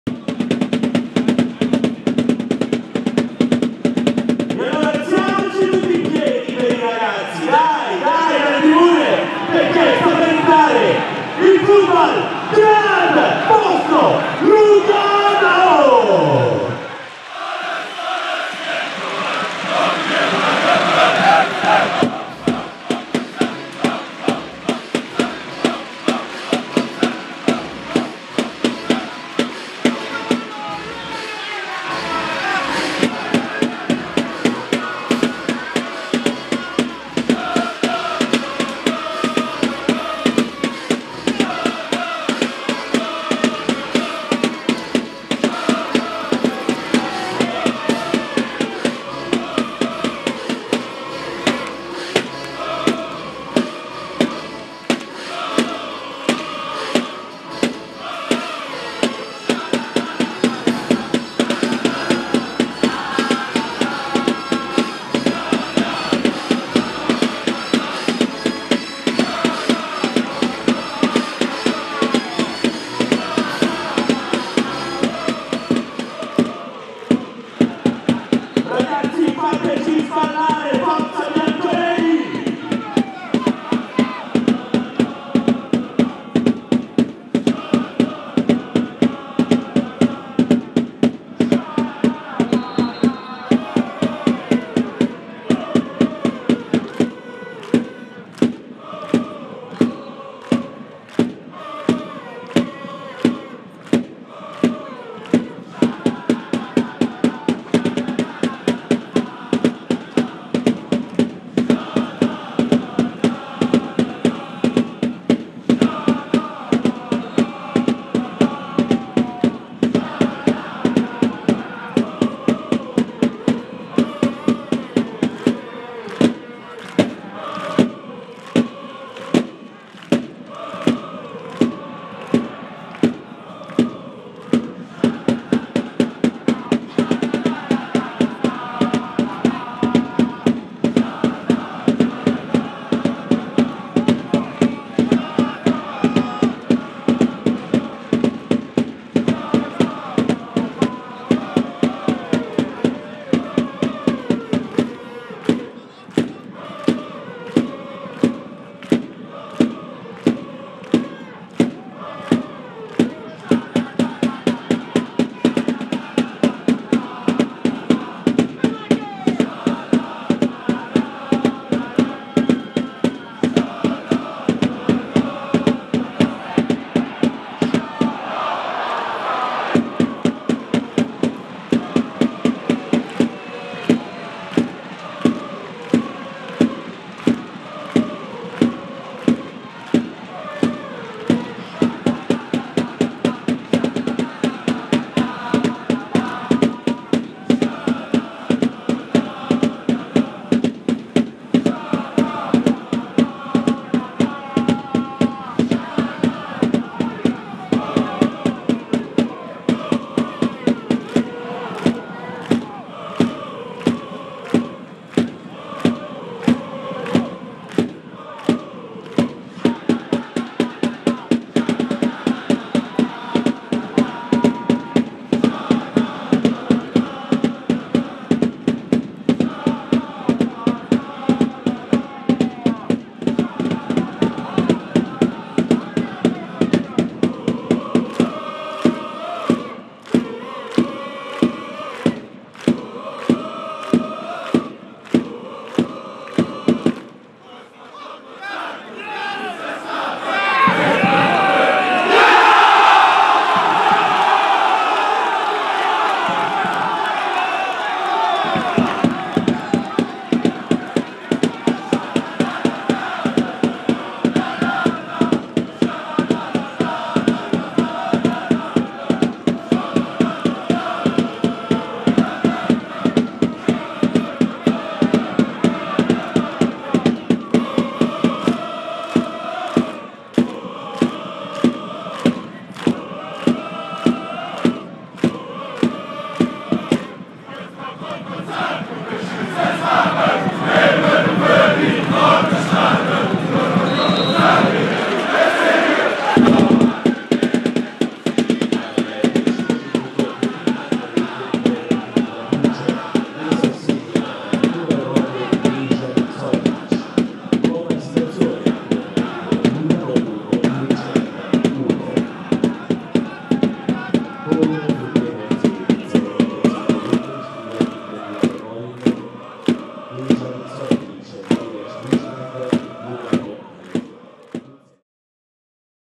SL 14. Runde: 21.11.2021, FC Lugano 3:1 FC Luzern
Cornaredo: 3'020 Zuschauer

Tore: 4' Reto Ziegler, 10' Dejan Sorgic, 35' Mohammed Amoura, 45'+2' Mijat Maric

Für jegliche Weiterverwendung (kommerziell und auch privat) des Videos bedarf es der schriftlichen Erlaubnis des Eigentümers (Via Impressum erfragen).